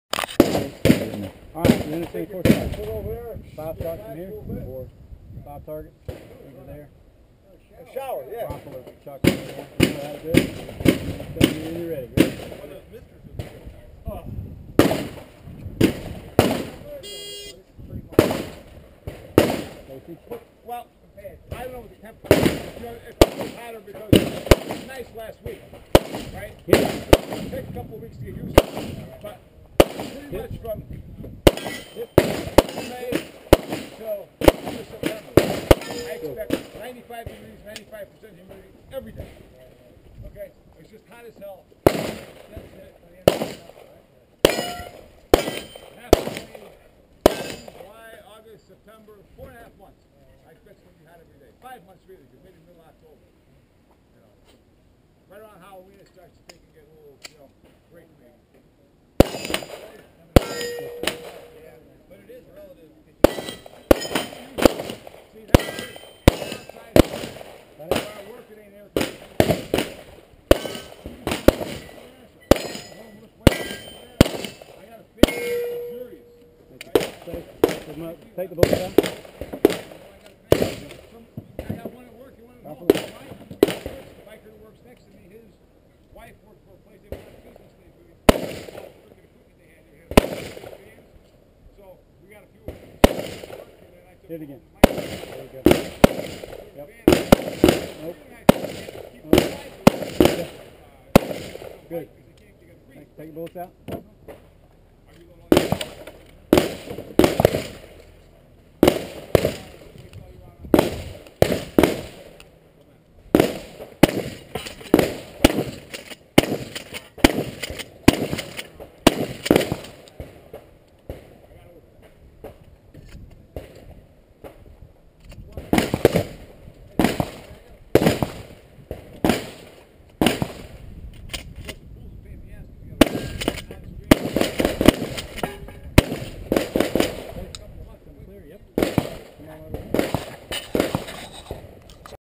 Scott Hernando 3Gun 042912
5 steal plates four times with pistol. Then free hand rifle at five clay targets on the berm. Last 10 shotgun targets.
Nice fun CoF. They should have more of these in the Summer months so the day isn't so long and miserable!